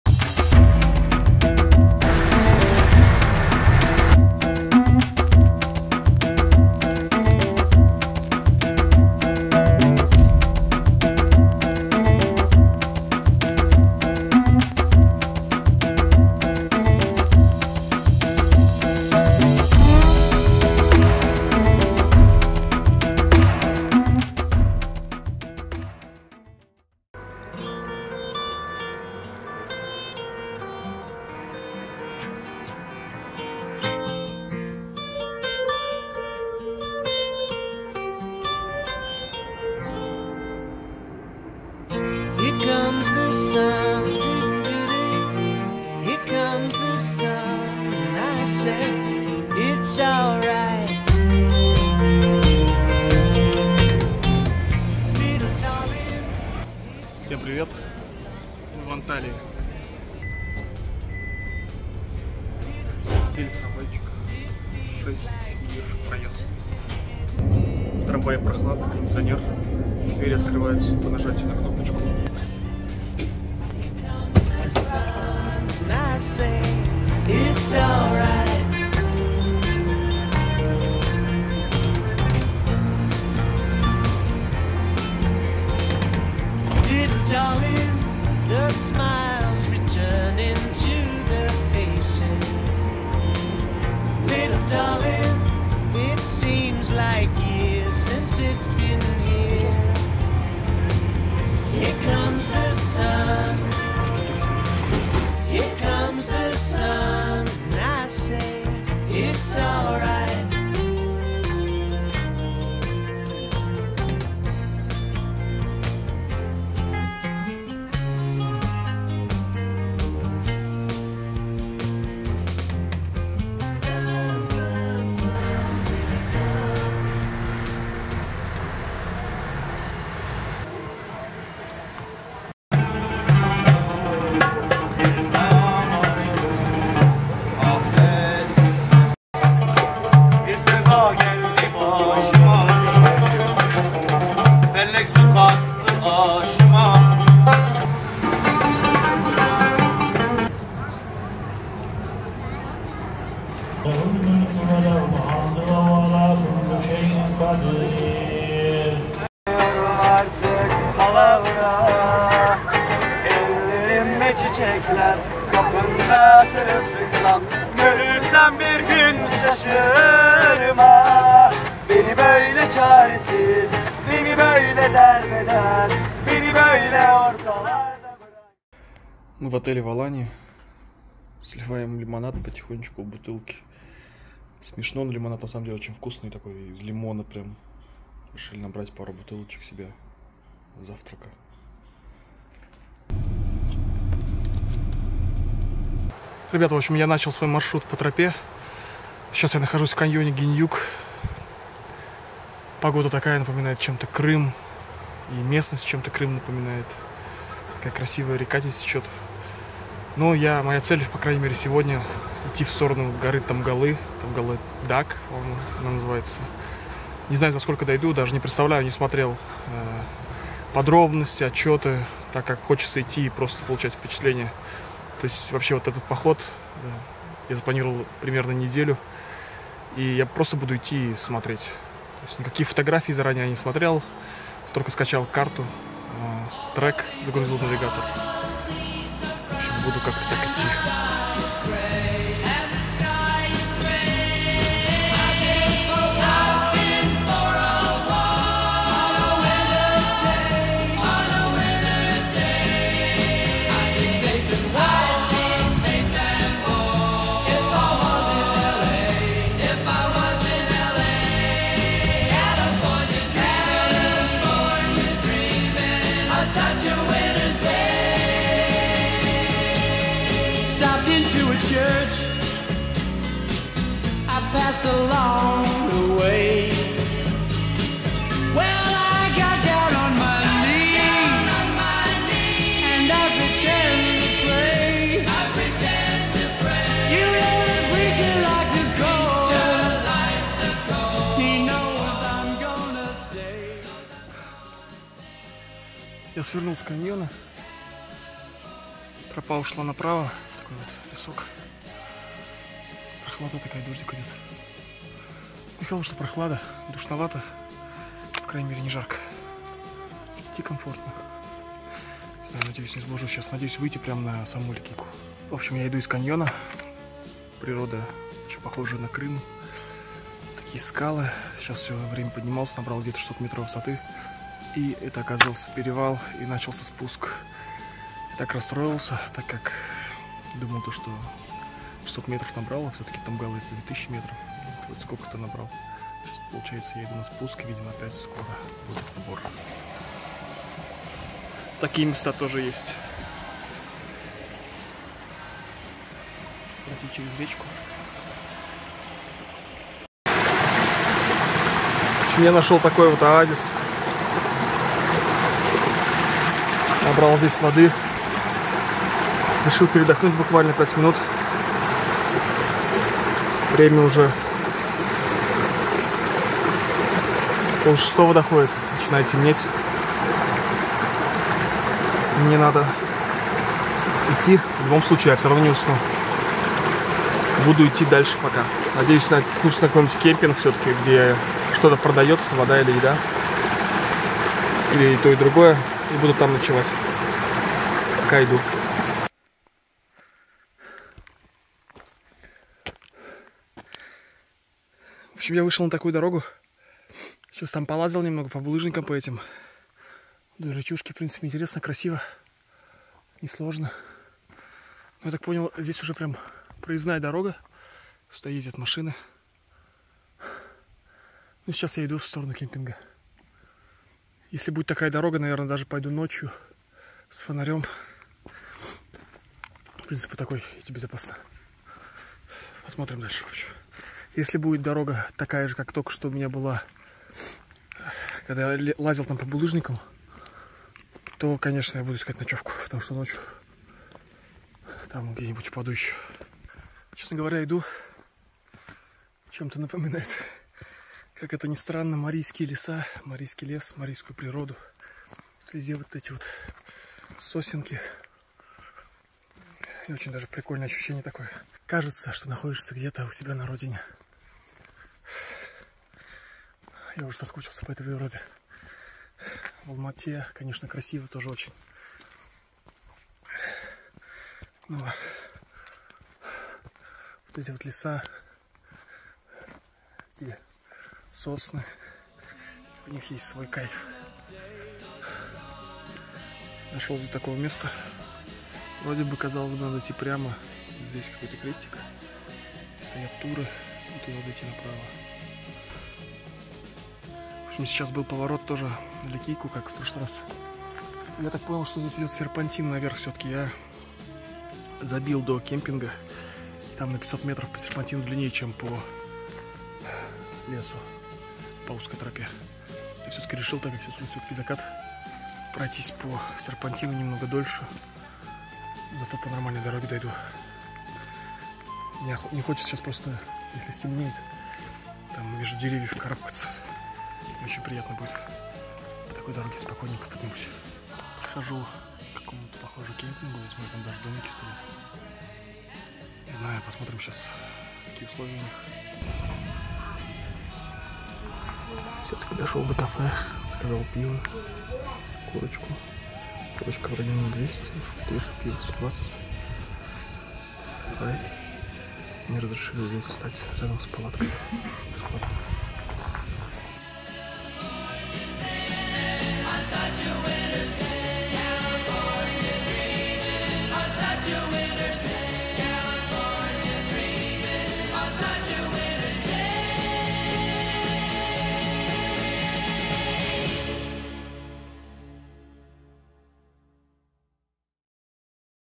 Один на Ликийской тропе. Часть 1. Анталия, Каньон Гейнюк.
Наше новое путешествие по Турции в начале октября. Посмотрели на Турцию совершенно с другой стороны и влюбились в неё. Вместо привычных отелей All Inclusive тут будет:
- путешествие по Восточной Ликийской тропе в одиночку
- каньон Гёйнюк
- огни Химеры
- ночевки в палатке
- перевал Тахталыдаг
- сказочный пляж Чиралы
- проливные дожди
- город Демре и древний город Мира
- город Каш с его потрясающим пляжем - Капуташ
- город Кемер
- удивительная Кападокия, Гёреме и полет на воздушном шаре
и никого не оставляющий равнодушным - Стамбул